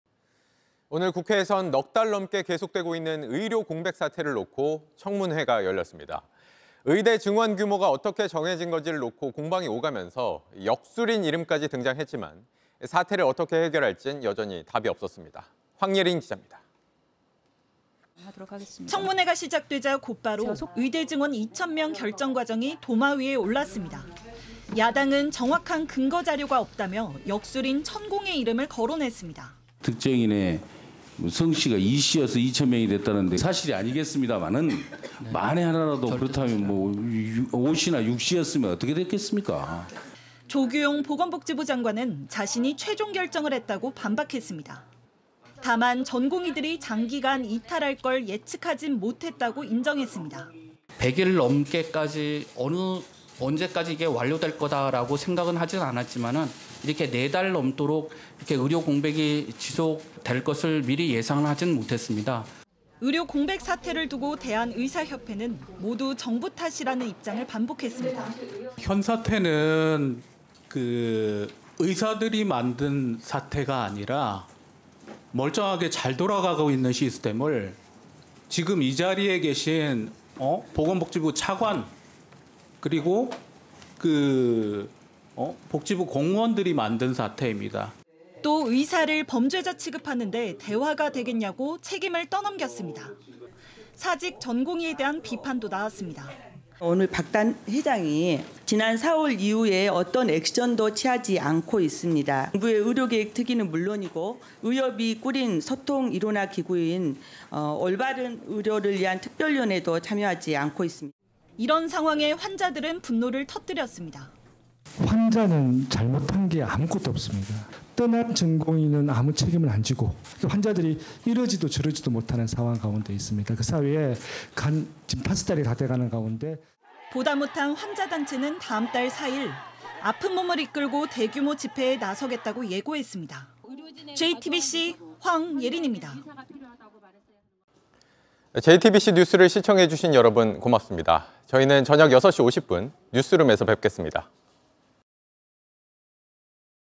"의대 2천명 증원 '천공' 때문인가"…국회서 의료공백 책임 공방 / JTBC 뉴스룸
오늘(26일) 국회에서는 넉 달 넘게 계속되고 있는 의료 공백 사태를 놓고 청문회가 열렸습니다. 의대 증원 규모가 어떻게 정해진 건지를 놓고 공방이 오가면서 역술인 이름까지 등장했지만, 사태를 어떻게 해결할지는 여전히 답이 없었습니다.